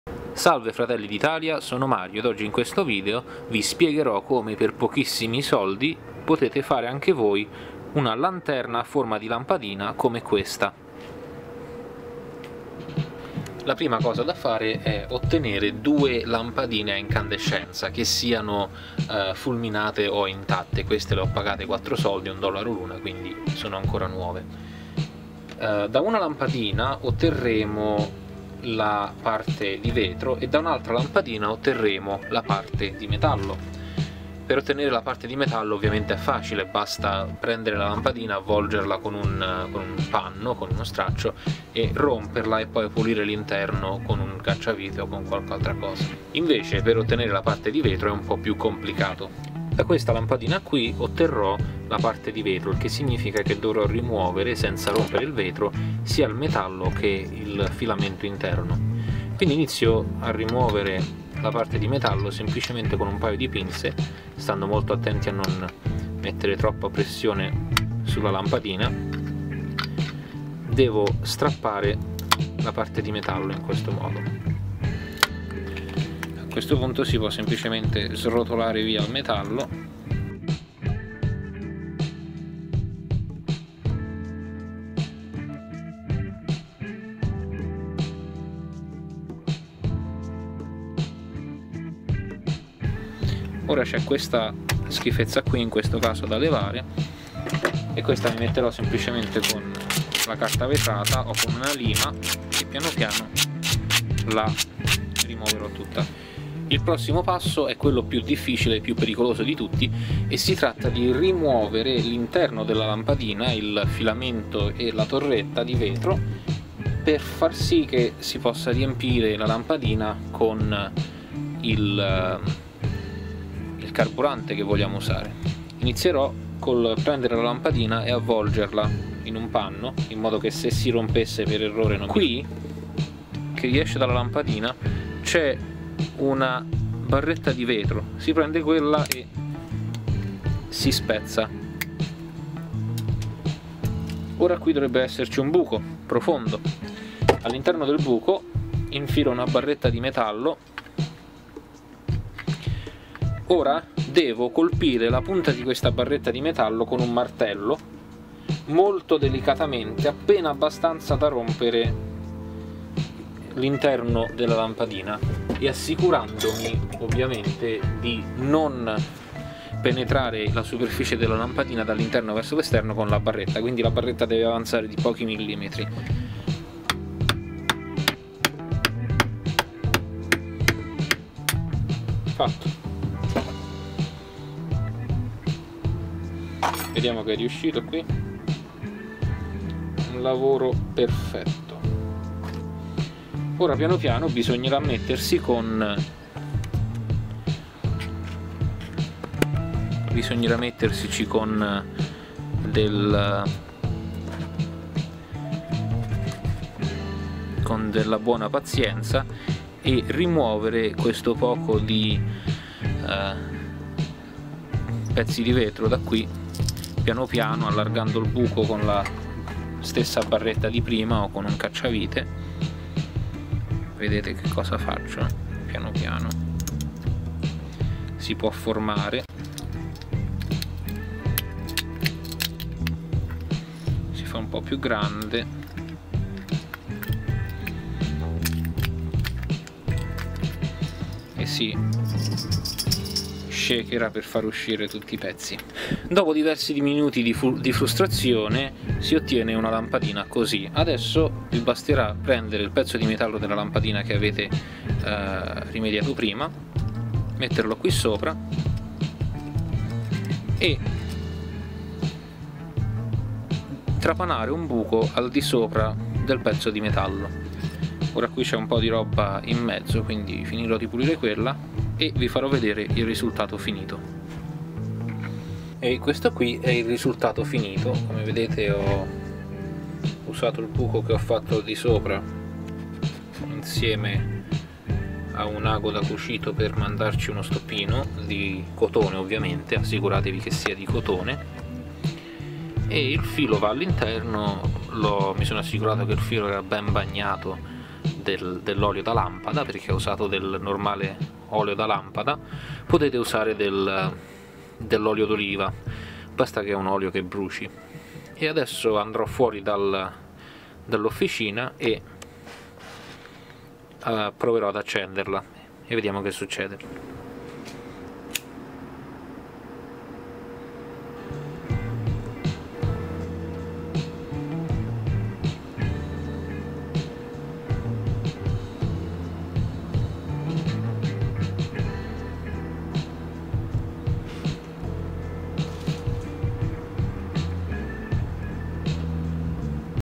Lampadina-lanterna ad olio fai da te
un gagget molto interessante. Tutorial: Come costruire una lanterna a olio a forma di lampadina